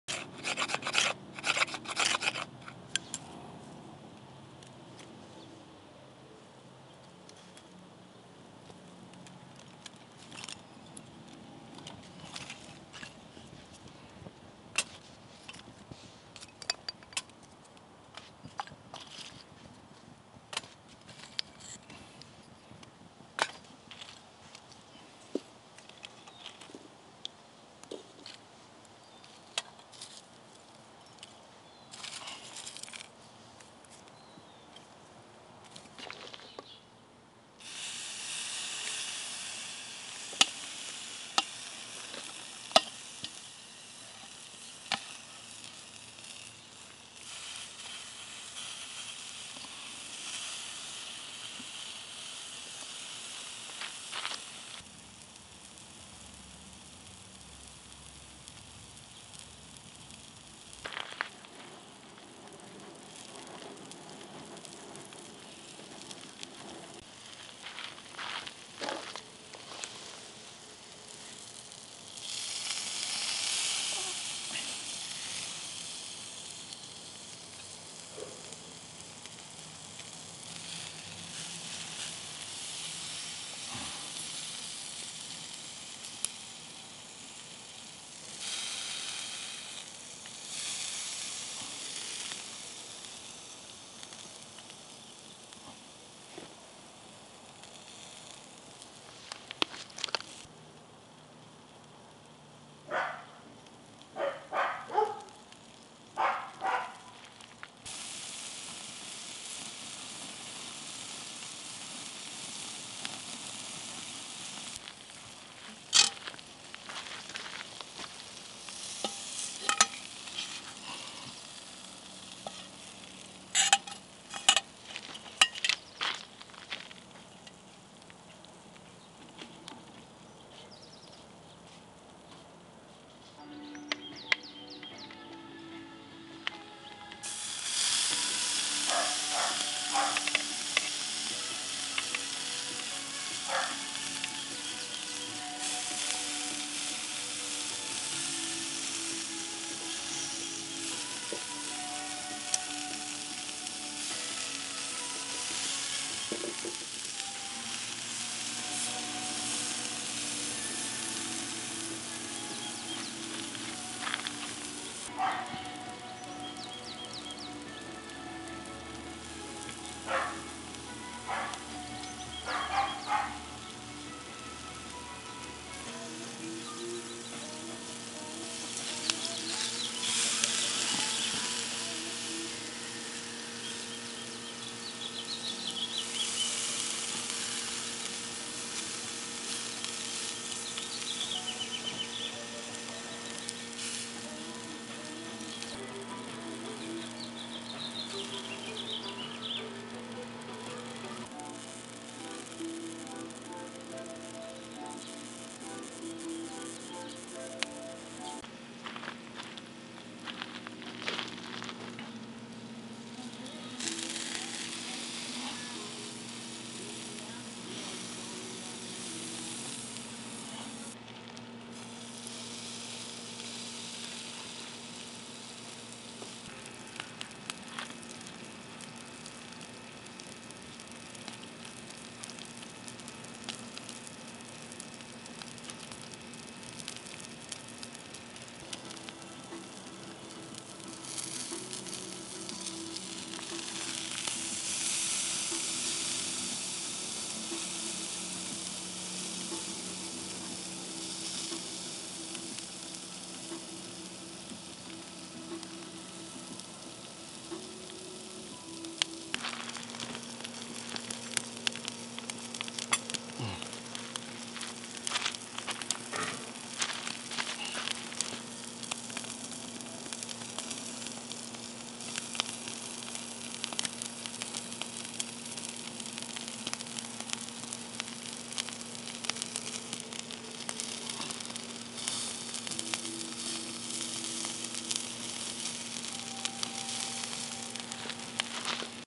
Grilling With A Firebox Stove. Pork Belly, Salmon, Filet Mignon
Grilling pork belly, salmon, and filet mignon using a firebox stove.